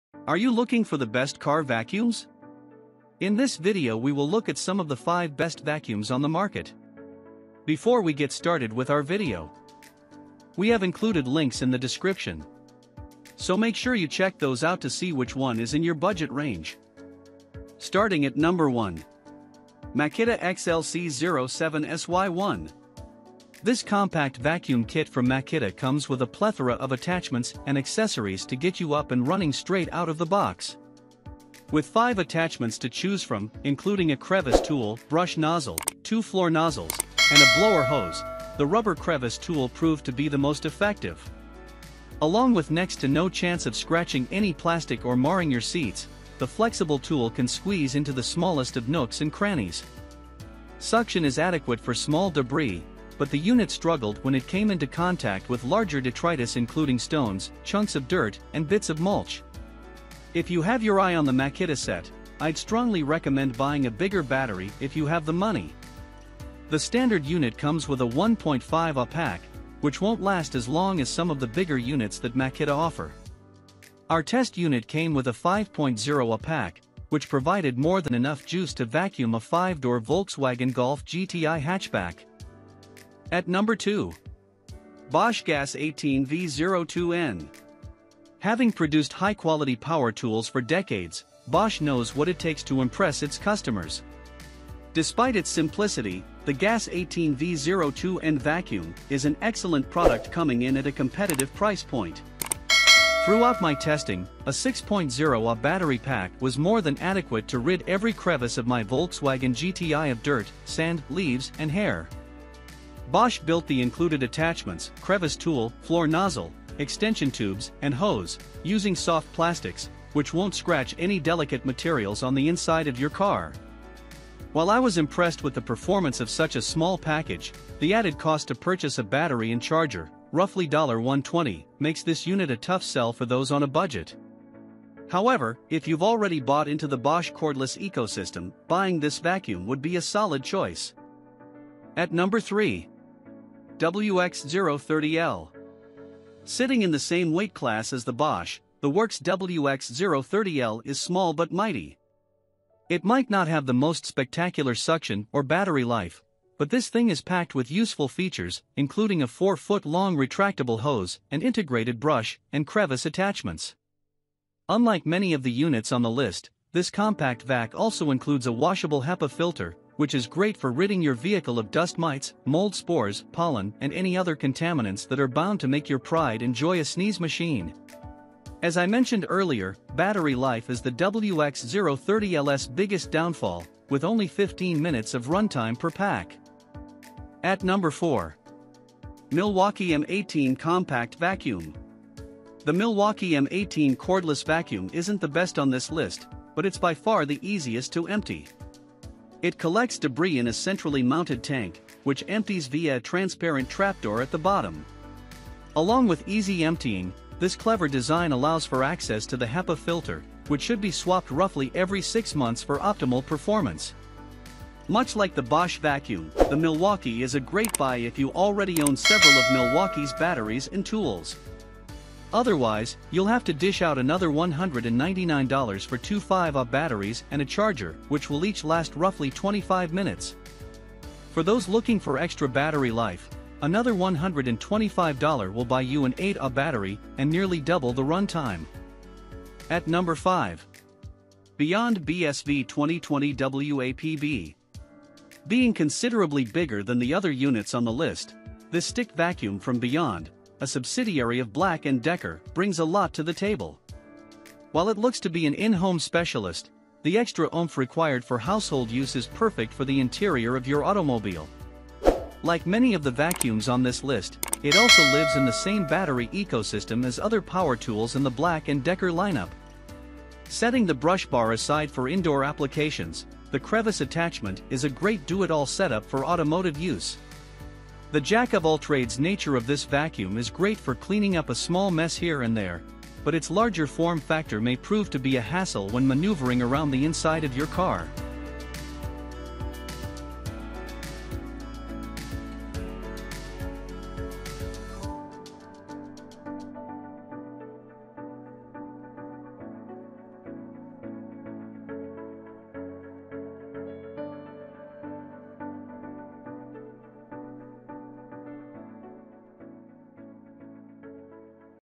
✅Best Car Vacuum Cleaner Reviews In 2022 | TOP 5 Best Car Vacuum Cleaner 2022. [ BEST REVIEWS TUBE ]
1. Makita XLC07SY1.

2. Bosch GAS18V-02N.

3. WX030L.

4. Milwaukee M18 Compact Vacuum.

5. Beyond BSV2020WAPB.

In this video, we listed the Best Car Vacuum Cleaner that are available on the market for their true quality, actually, I tried to make the list based on their popularity quality-price durability user opinions and more.